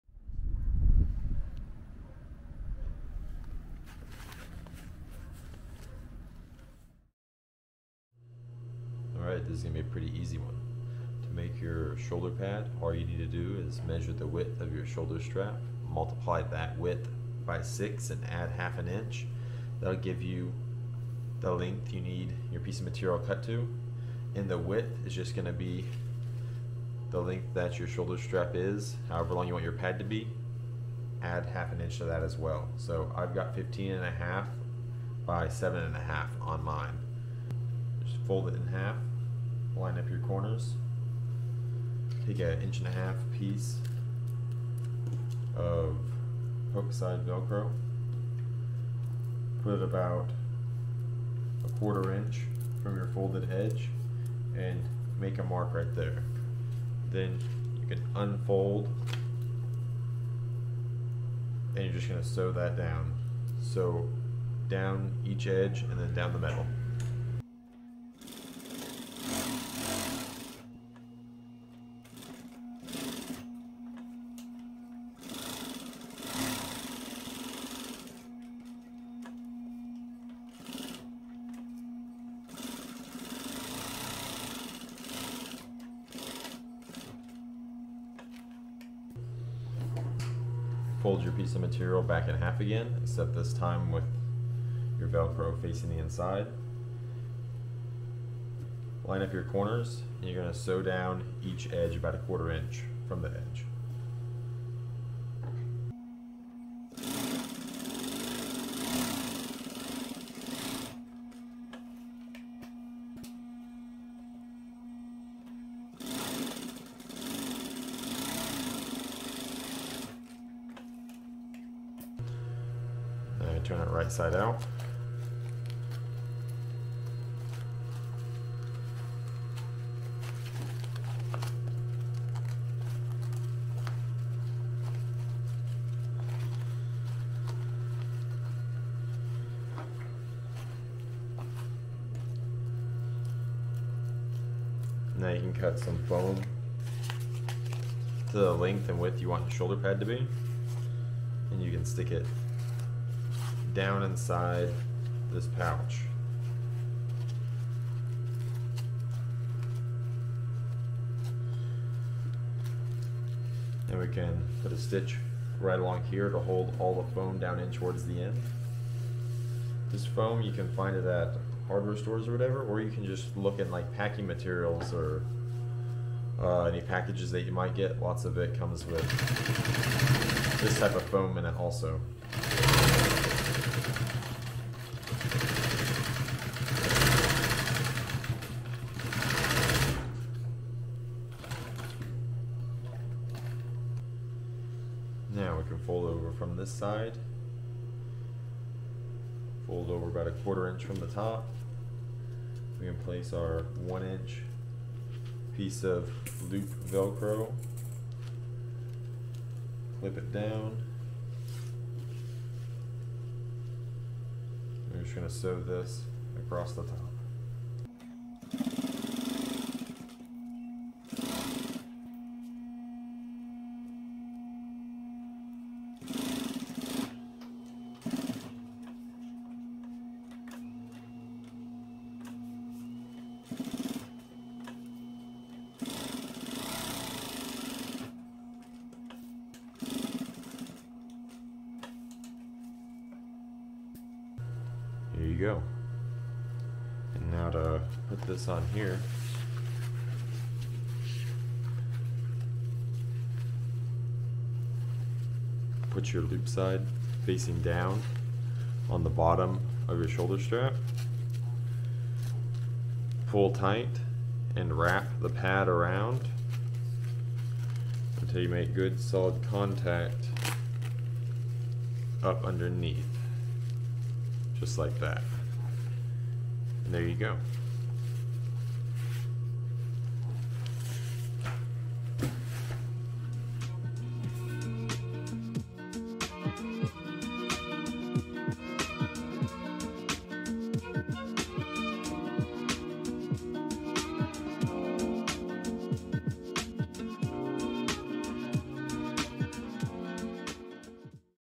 How to make shoulder pads for your chest rig or plate carrier
How to make a low profile set of shoulder pads for your chest rig or plate carrier.

Cheap Sewing Machine
Better sewing machine
1000D Cordura Nylon
Bonded Nylon Thread
2” Velcro
DJI Pocket 2
Telescopic Tripod